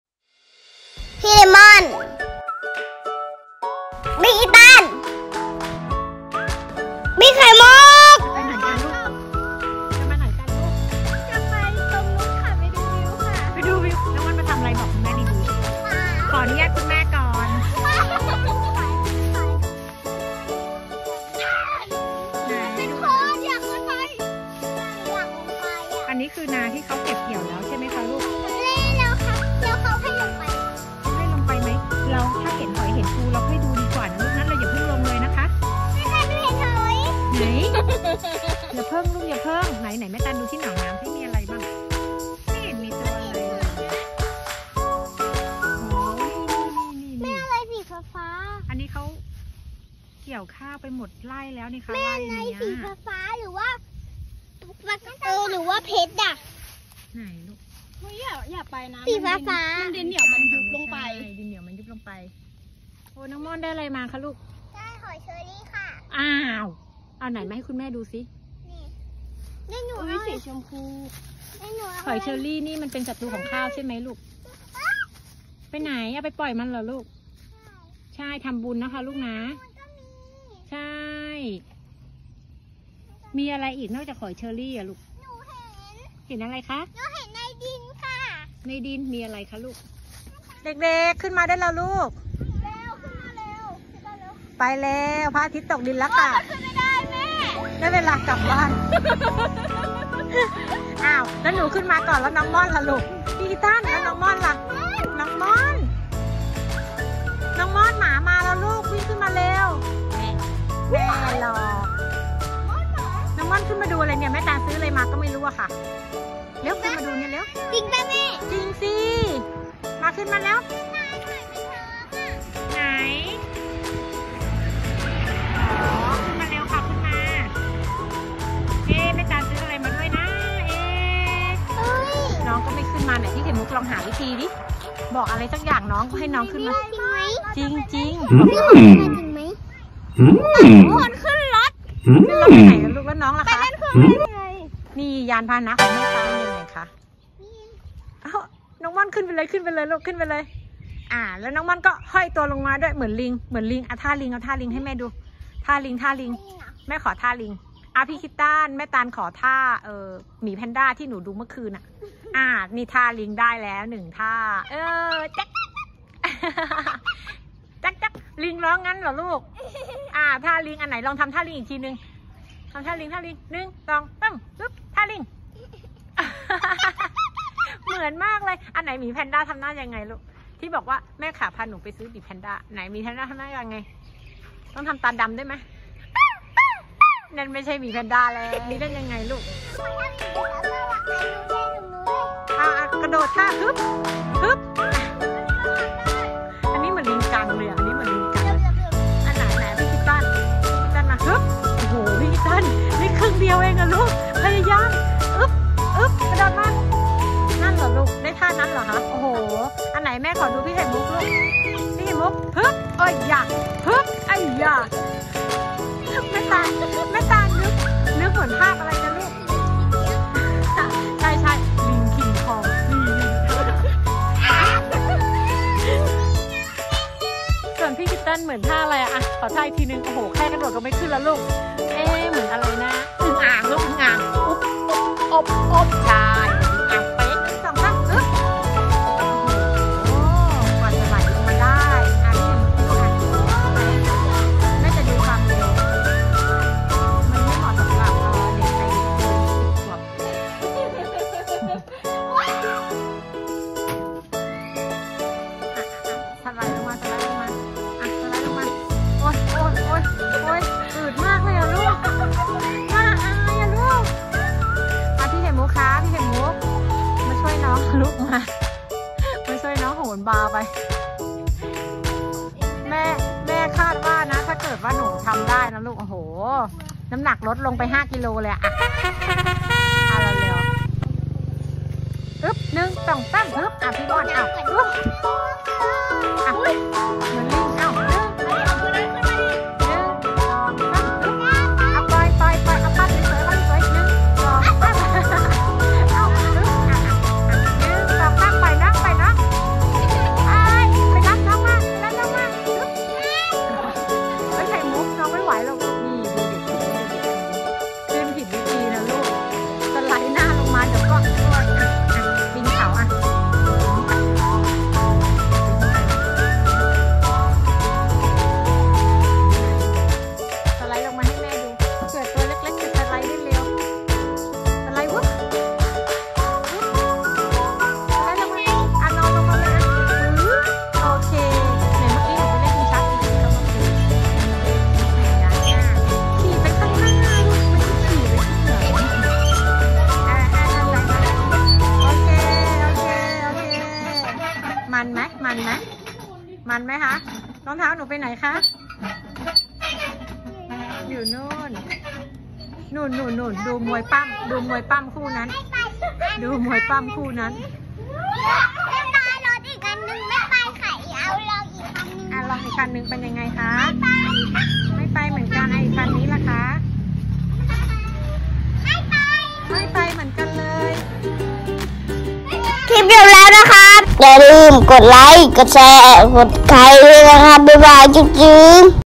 พี่คริสตัล น้องไดมอนด์ | พาเที่ยวชมธรรมชาติและเล่นเครื่องเล่น อู่ทอง สุพรรณบุรี//Funny kids
พี่คริสตัล น้องไดมอนด์ | พาเที่ยวชมธรรมชาติและเล่นเครื่องเล่น ที่โรงเรียนวัดคณฑี อู่ทอง สุพรรณบุรี

ยากชมคลิปอะไรคอมเม้นท์ได้นะคะ
ขอบคุณที่ติดตาม และเป็นกำลังใจนะคะ